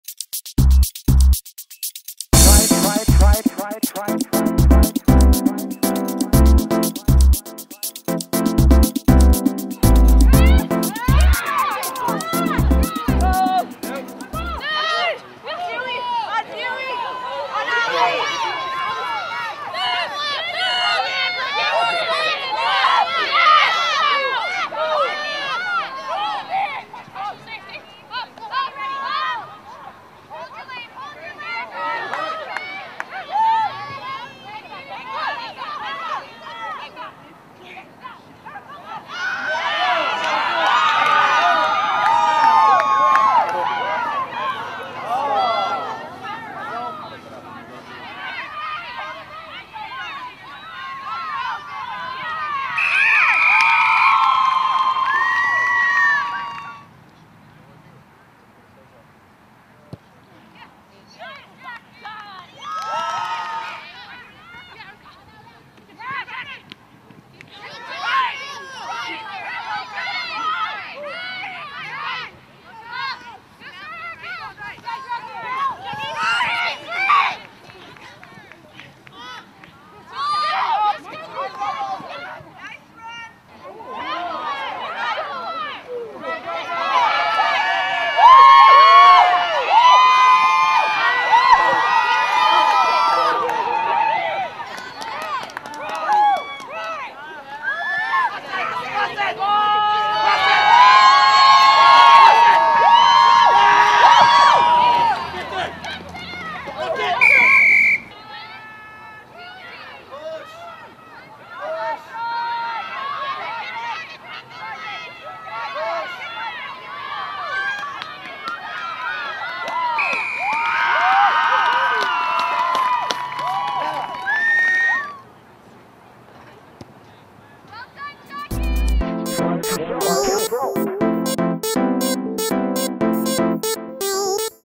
Stingers Women's Rugby vs. Gee-Gees
Concordia Stingers Women's Rugby vs. Otawa Gee-Gees

Game Highlights

Concordia Field, Montreal

Video by Joana Rodrigues